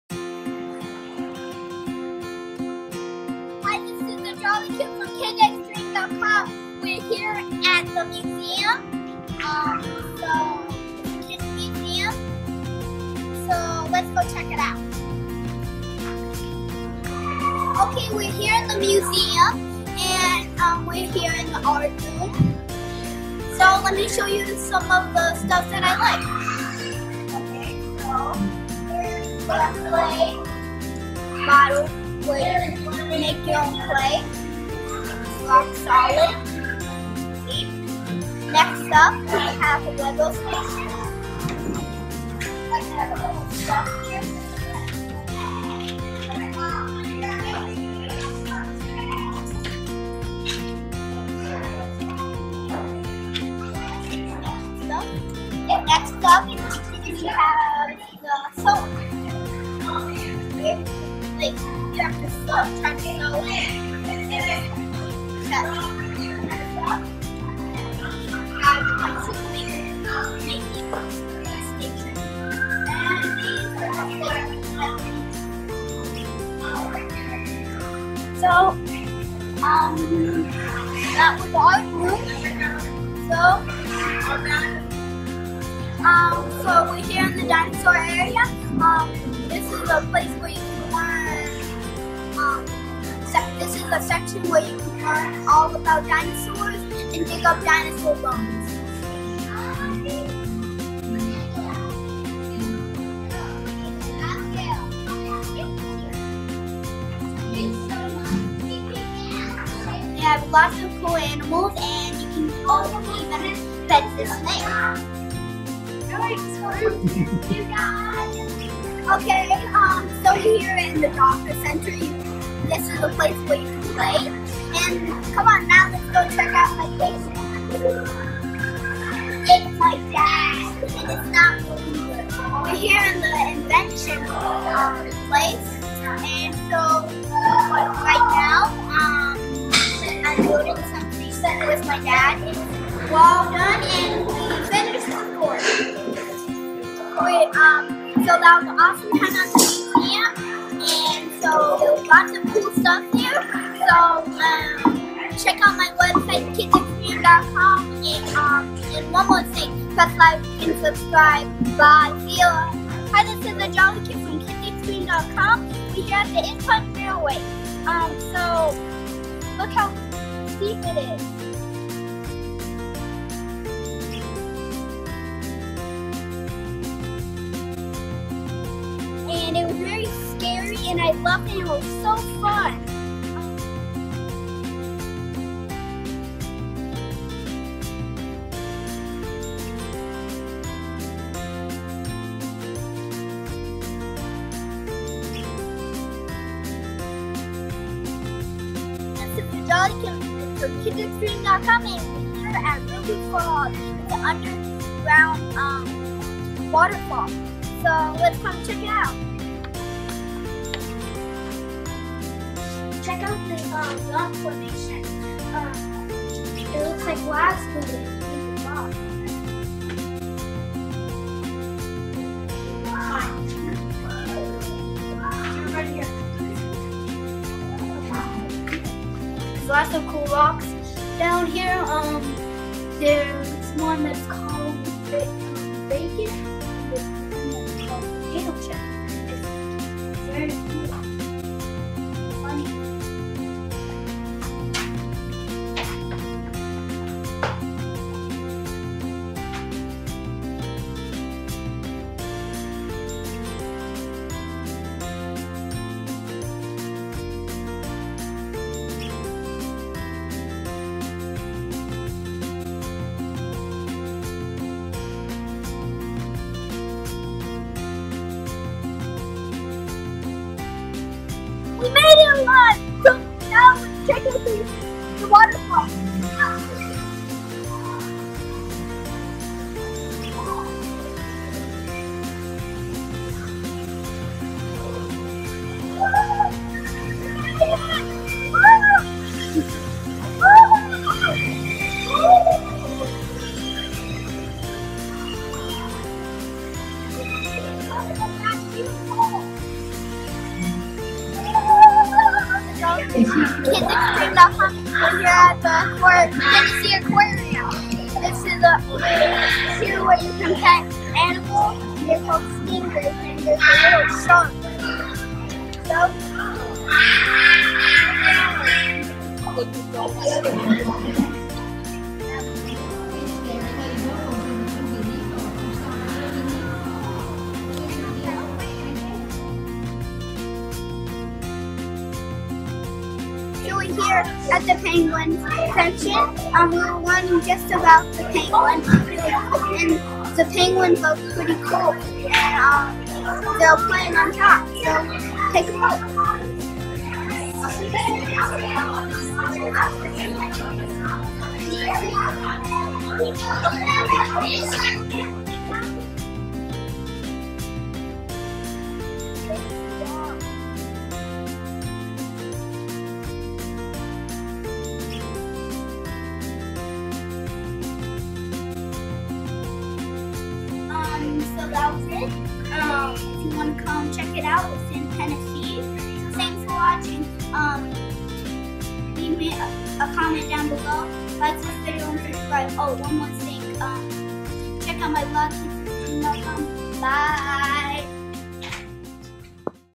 Things To Do with Kids in Chattanooga, TN | KidsXtreme
Check out the funnest places to visit with kids in Chattanooga, TN! for more videos and tips for kids by kids!